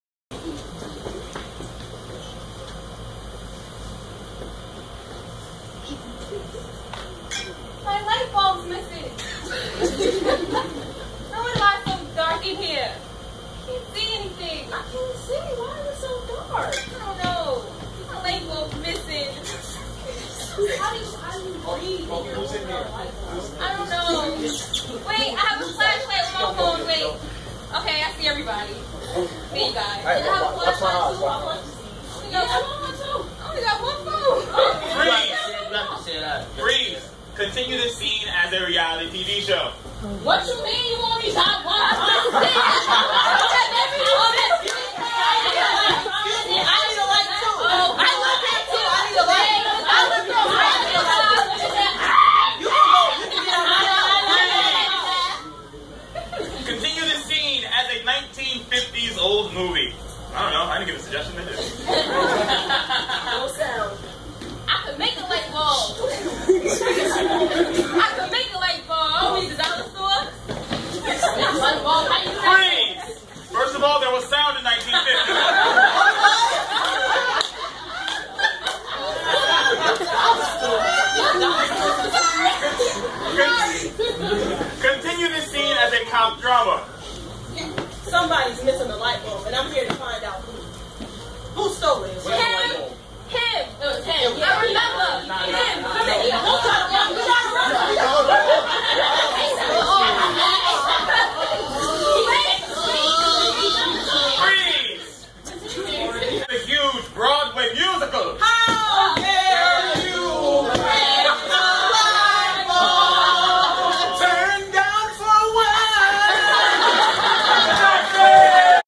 "Styles" - January 2014 Show
Improv students tell the dramatic story of a broken lightbulb through different entertainment genres.